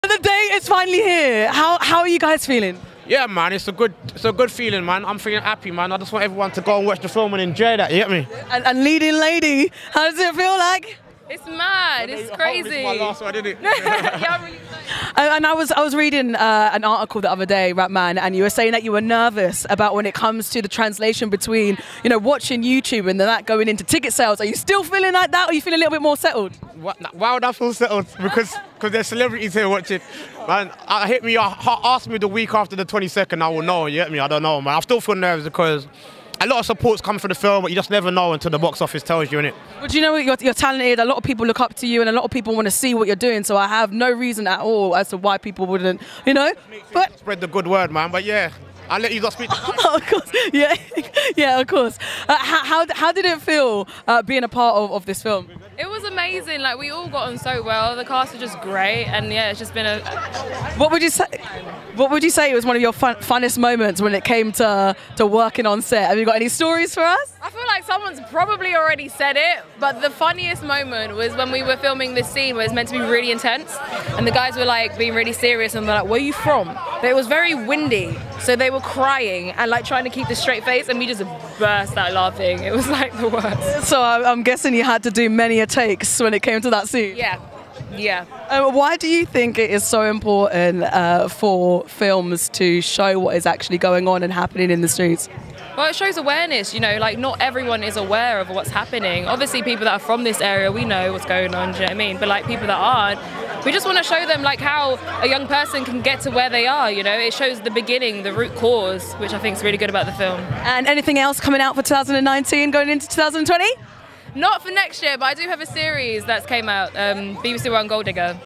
Jourds speaks to Karla-Simone Spence & Rapman at the Blue Story Premiere
Presenter @Jourds_
Guests @KarlaSimonee @RapMan
Filmed & Edited By @Jojobami